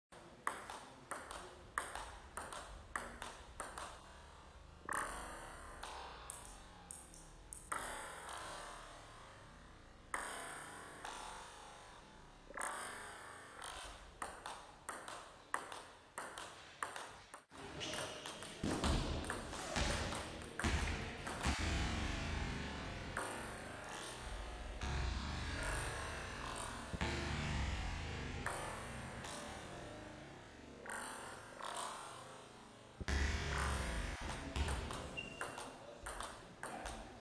RH Schupf (9)
Es wird ein Einblick in verschiedene Grundschlagarten und deren didaktische Einführung mit ausführlicher Technikbeschreibung sowie Tischtennisgrundwissen gegeben.